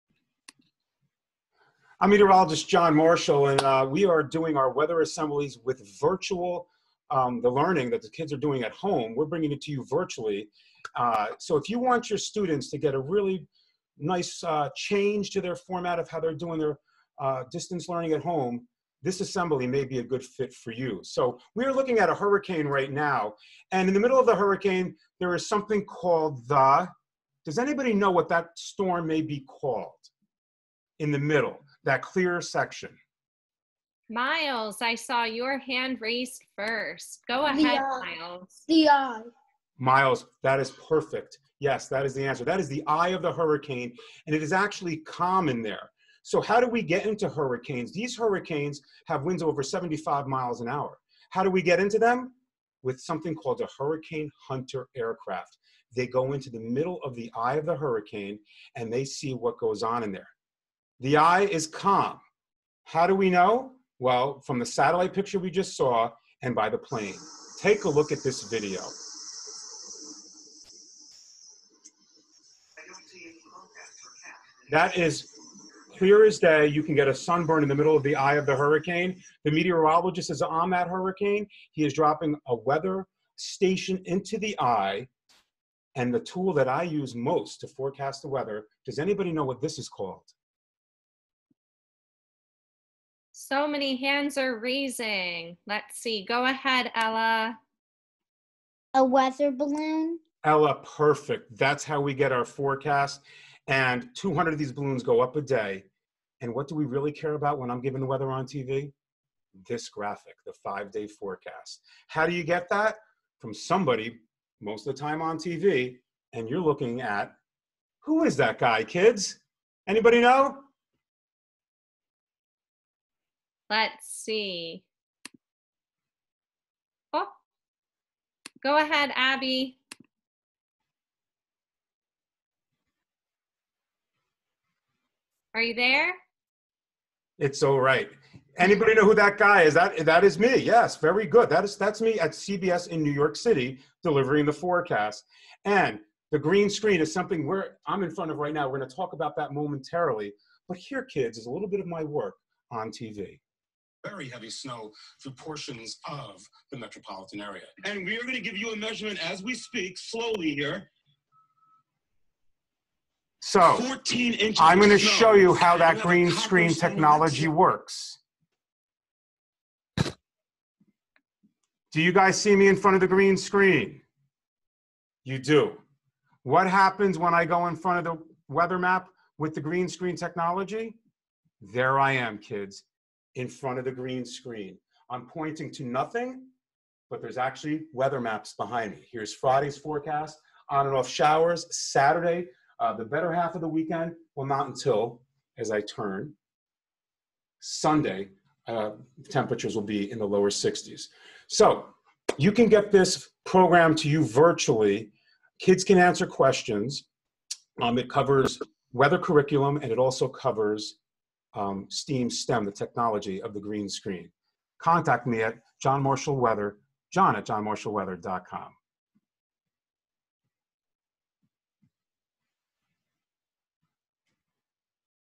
Virtual Weather Assembly Preview
It would be my pleasure to present the virtual weather assembly to your students. The students get to see how TV weather gets done with the green screen live!
Also age appropriate curriculum is part of the program. The students can ask questions, etc. This is all done via Zoom.
Hop aboard the virtual weather assembly train!